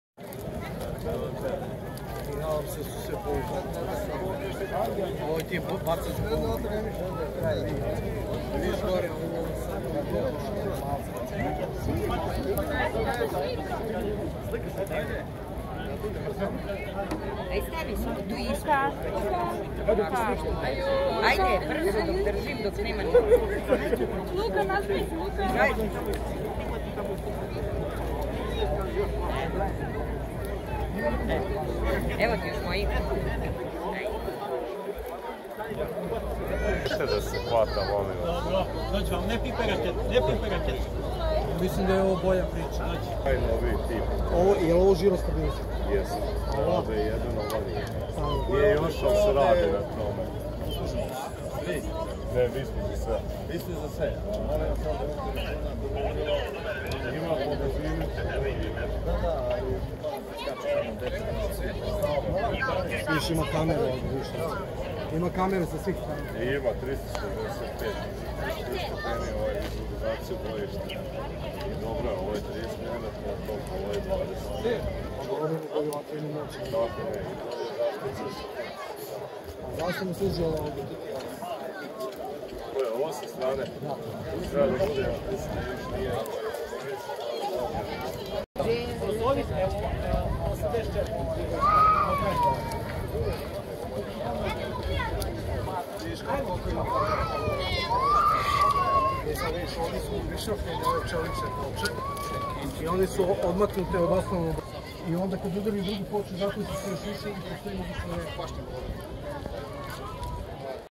Vojska Srbije: Modernizovana borbena vozila pešadije BVP M-80AB1 i BVP M80A
Vojna smotra Zastava 2024.